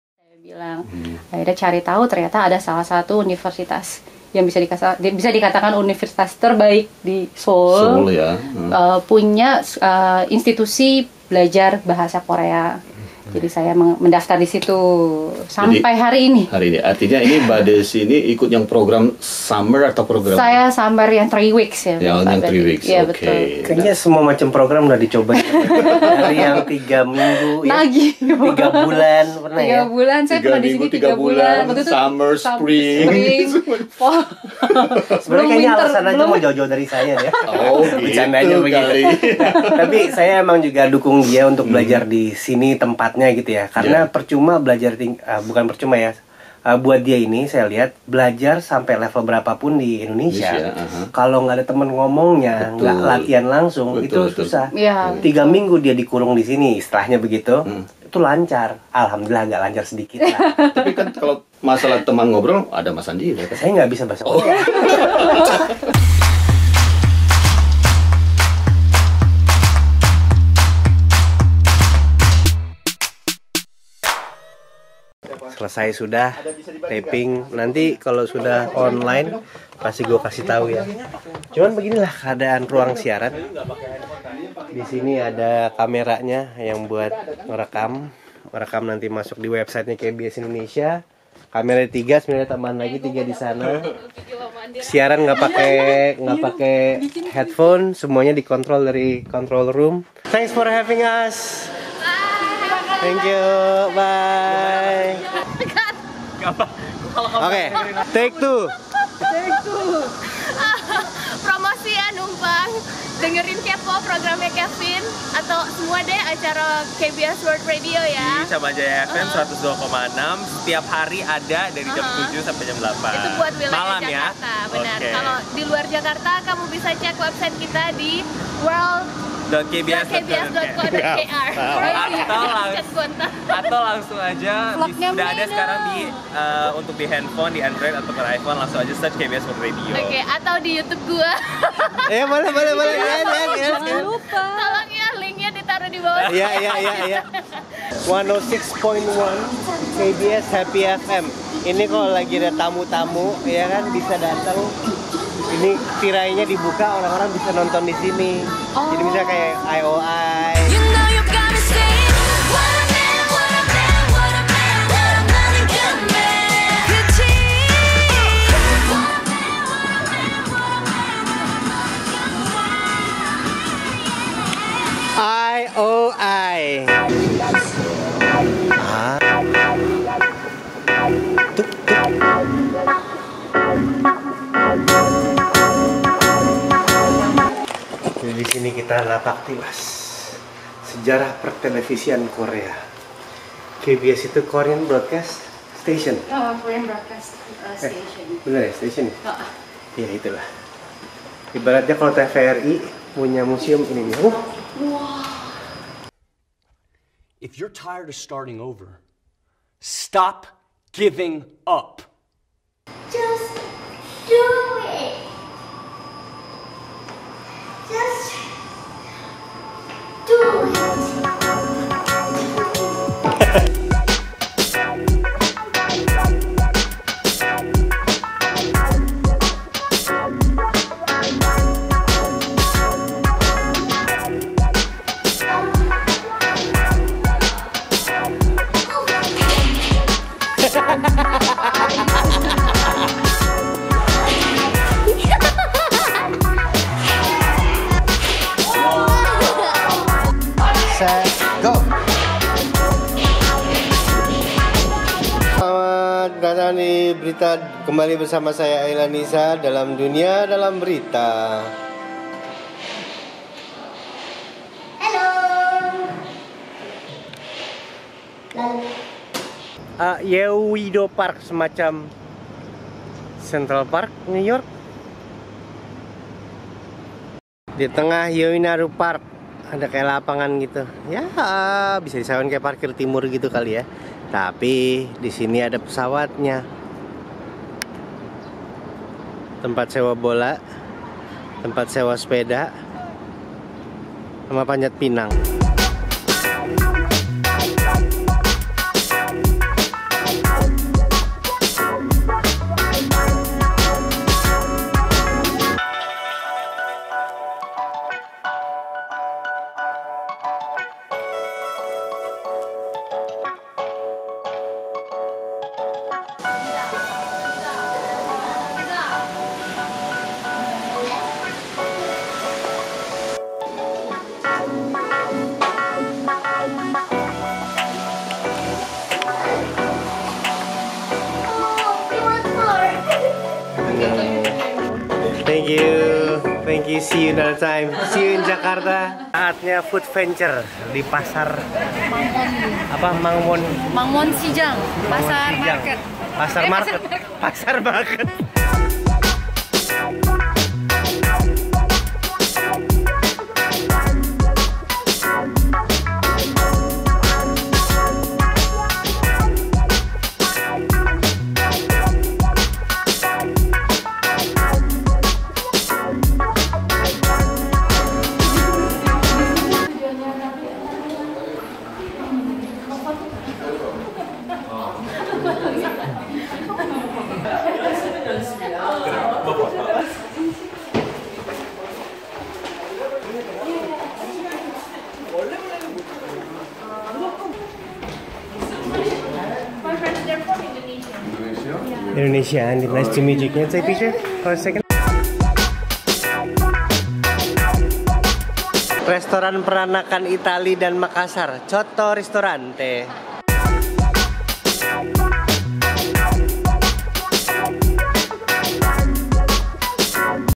Siaran di KBS World Radio
Setelah lama (pake banget) udah gak edit series vlog Korea 2016, akhirnya kita mulai lagi. Ini adalah waktu gue, Deasy, dan Isla diundang siaran di KBS World Radio, Seoul. Berbagi cerita kenapa kami bertiga ada di Seoul selama satu bulan full.

Lumix G7
Panasonic 12-35mm f.28
Samsung S7

Let it out, create!